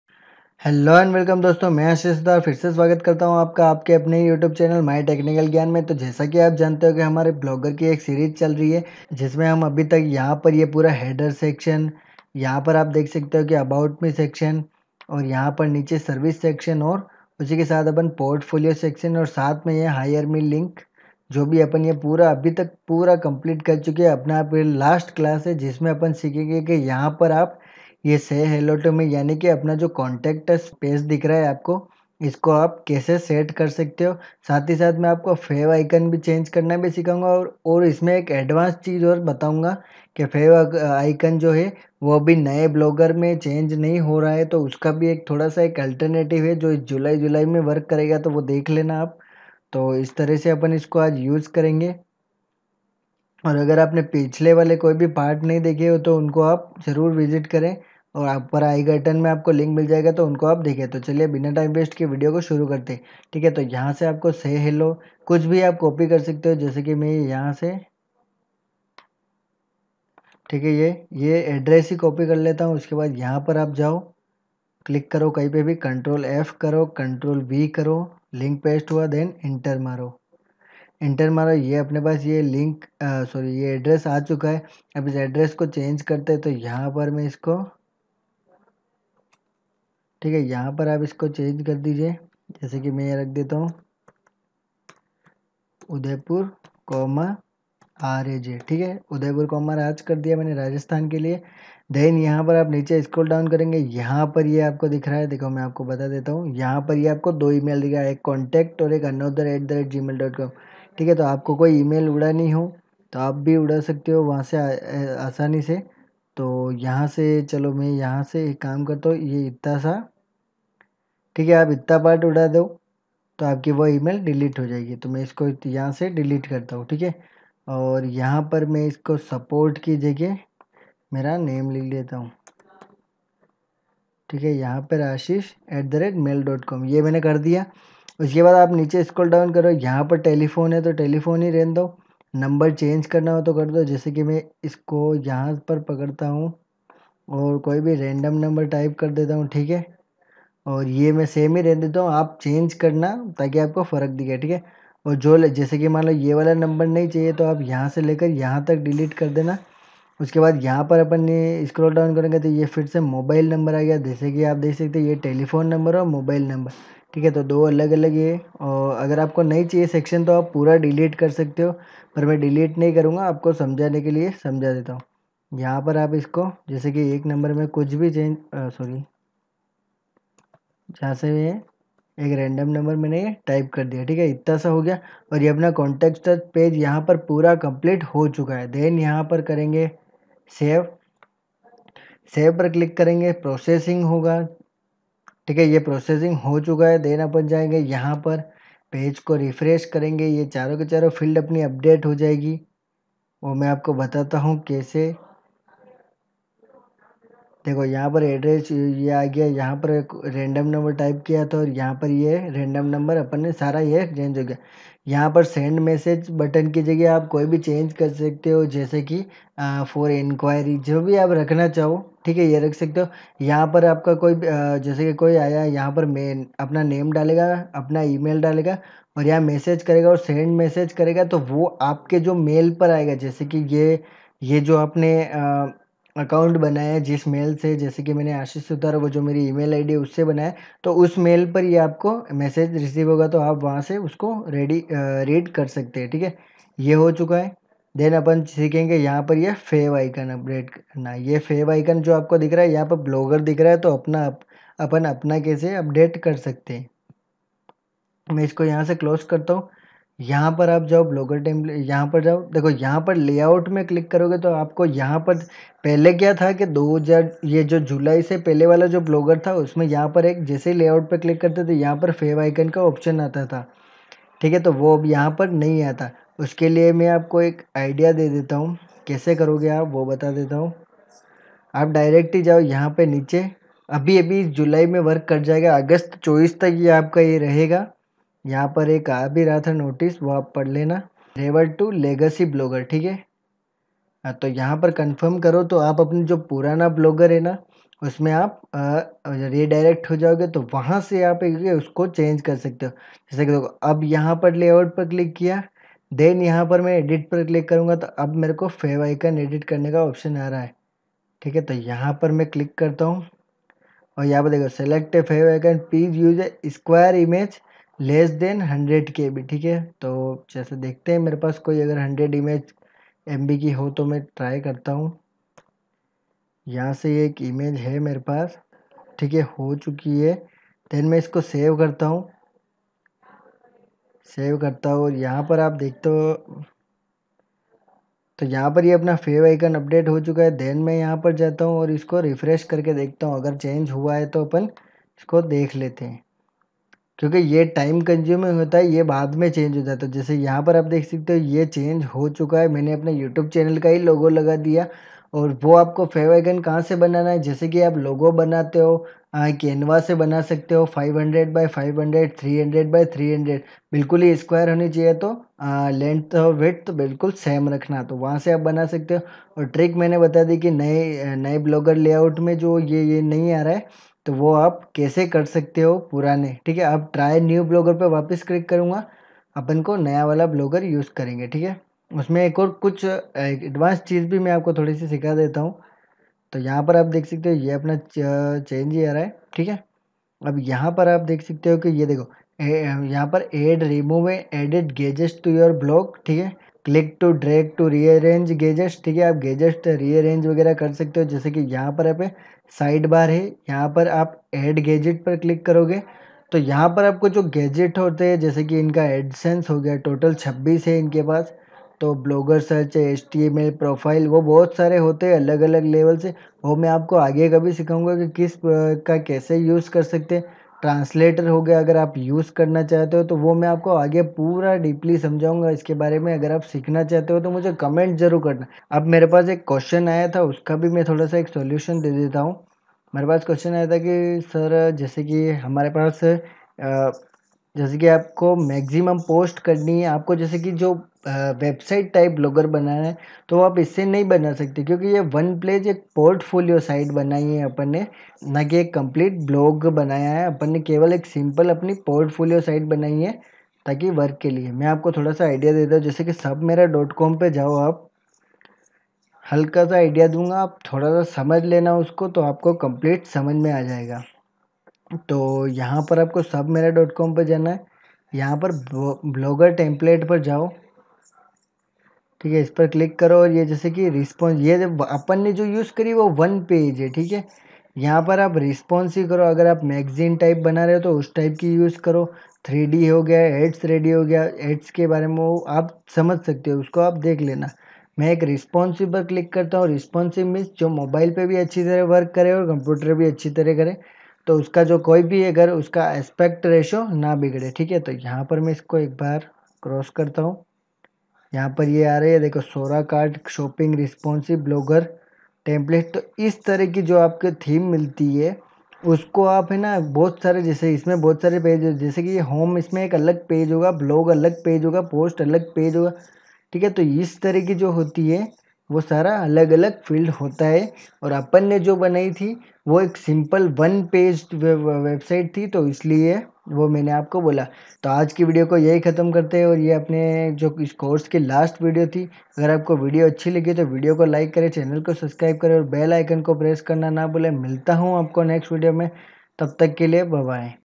Blogger Contact Page Customization | Change Favicon | Make Money Online | my technical gyan
HELLO FRIENDS,
इस video में मैने आपको बताया है की आप contact page कैसे setup क्र सकते हो कैसे आप favicon change कर सकते हो.